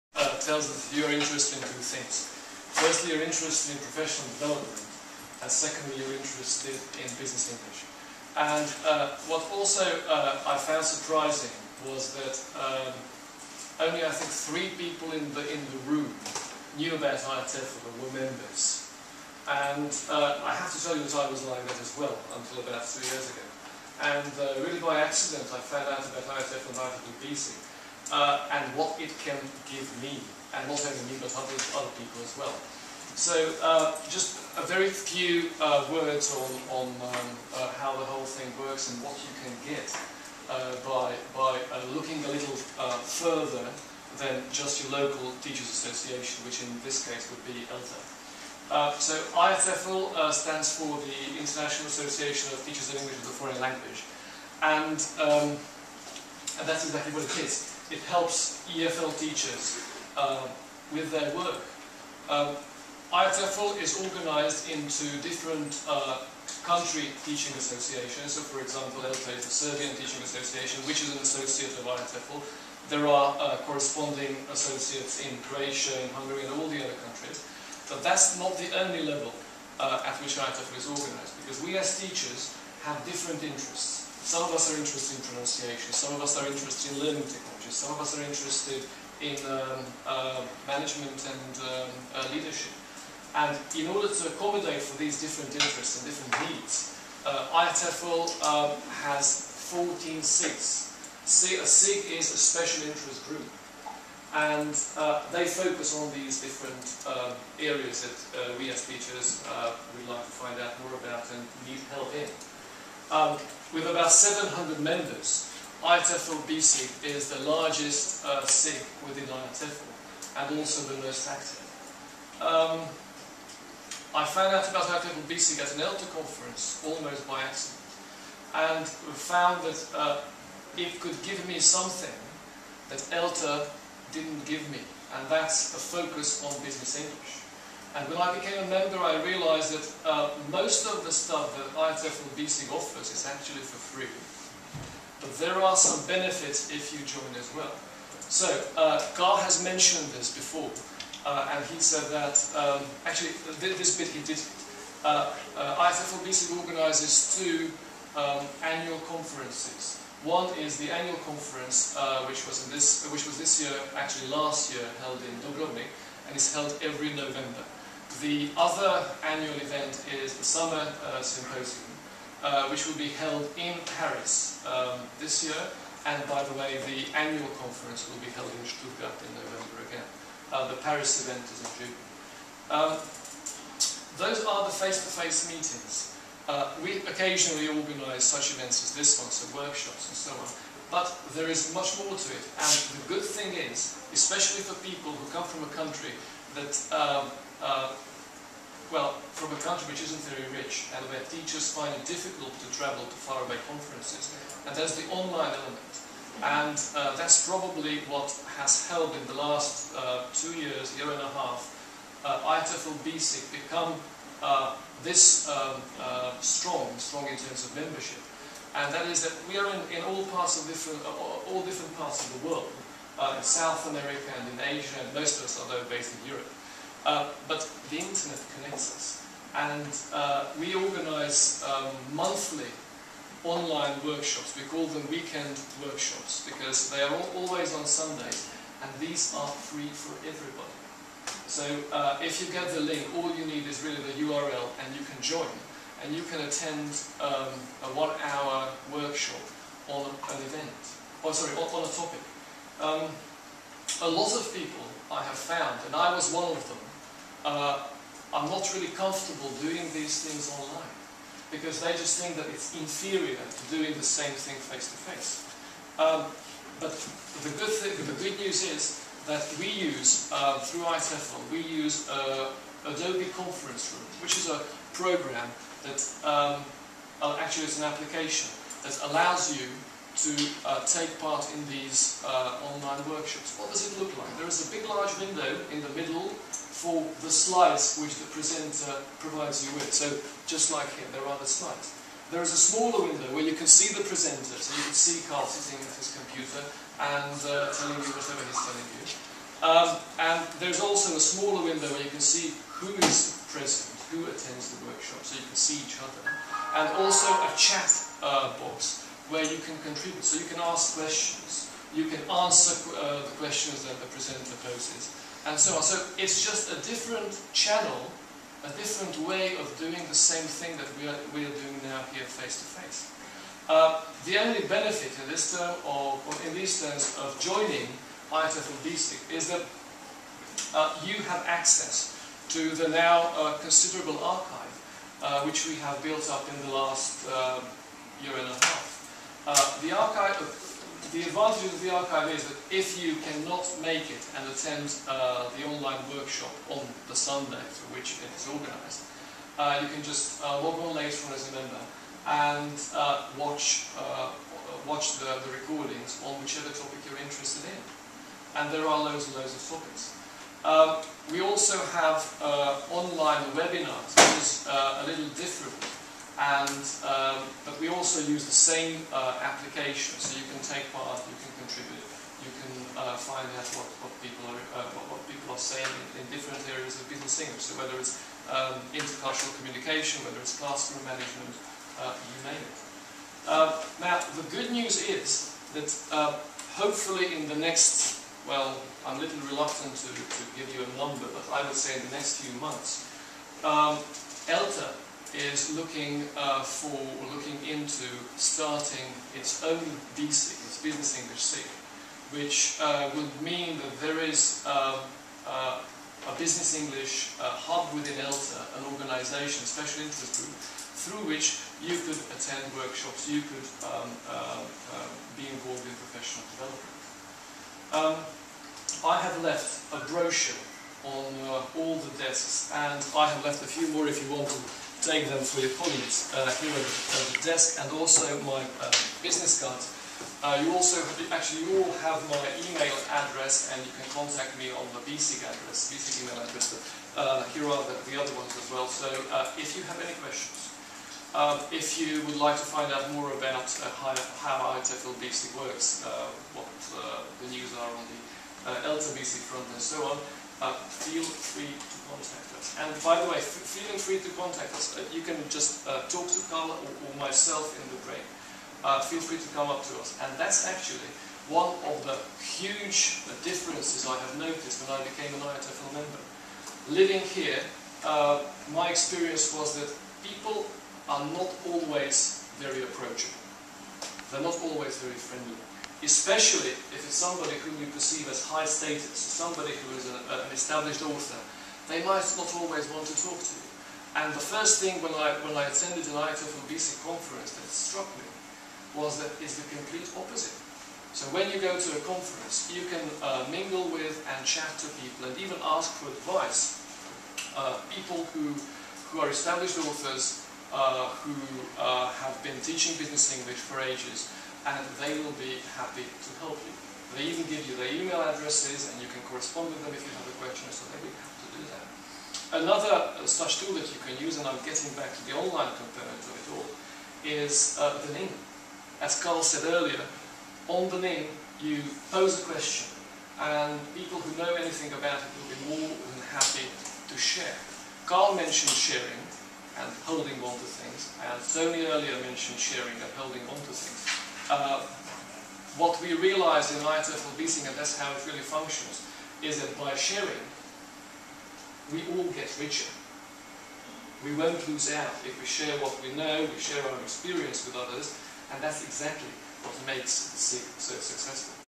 IATEFL BESIG Workshop in Belgrade
IATEFL BESIG Development Coordinator Akos Gerold addresses the teacher attending the workshop run by fellow BESIG Committee member Carl Dowse.